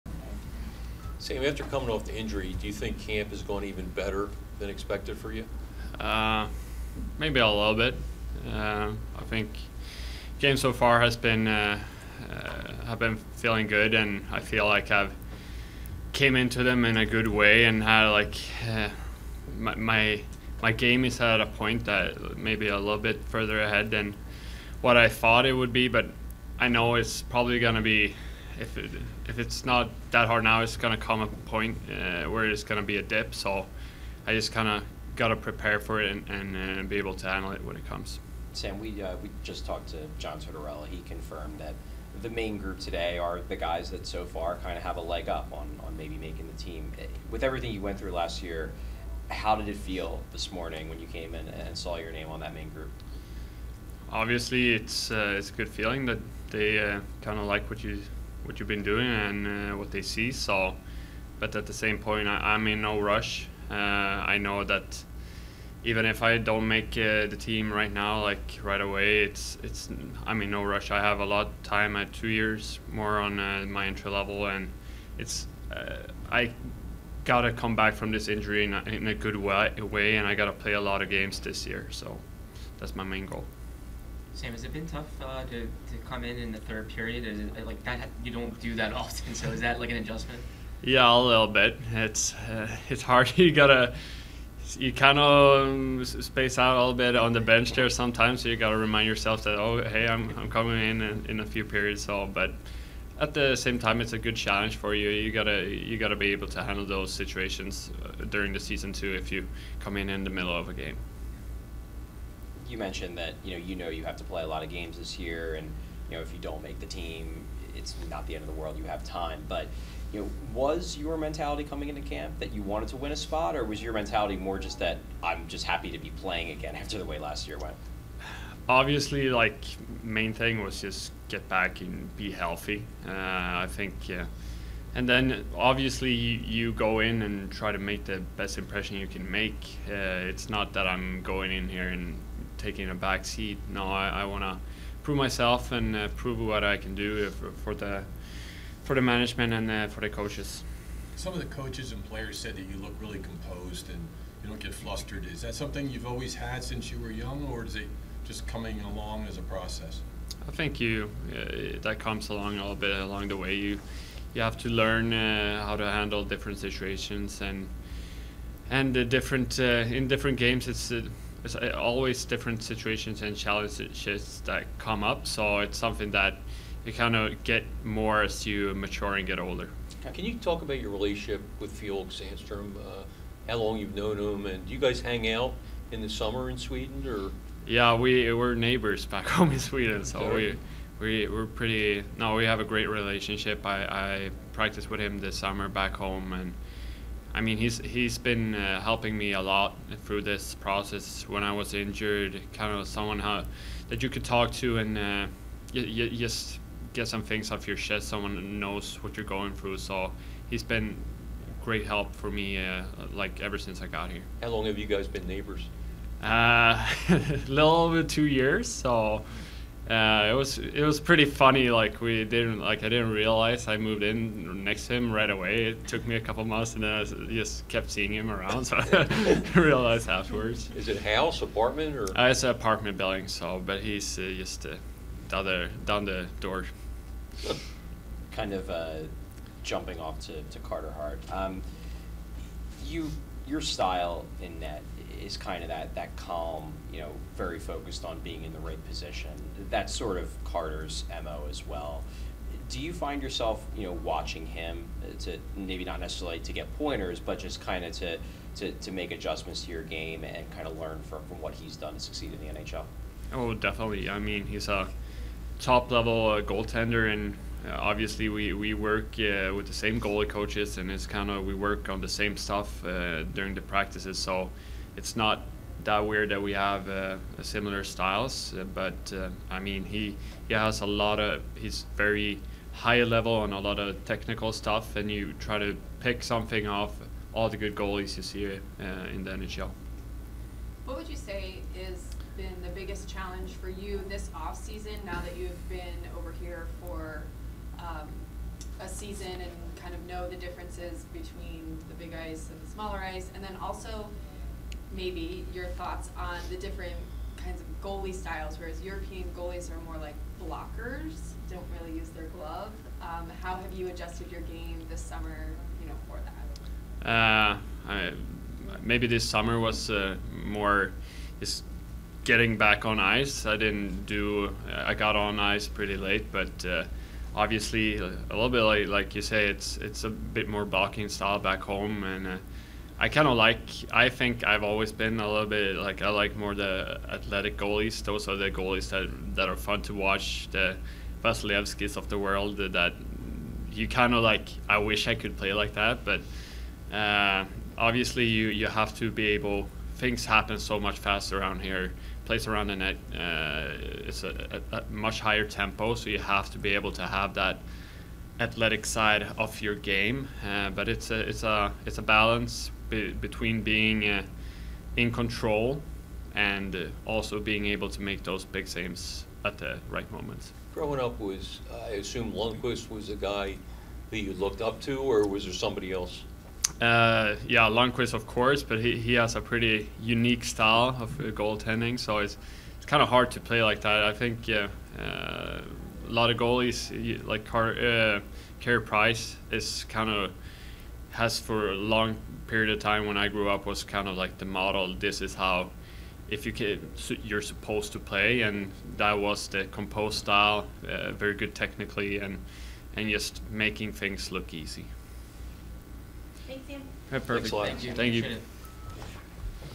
9/30 Training Camp: Samuel Ersson
Flyers goaltender Samuel Ersson meets with the media from Flyers Training Center.